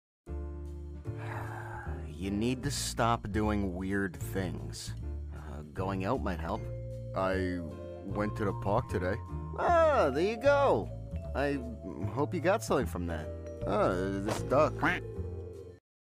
Sebastian finds a Frog (or duck?) | Stardew Valley Animation
Ive been binge playing stardew for like 2 weeks straight and sebs is my husbando so,,,
yeah this happened.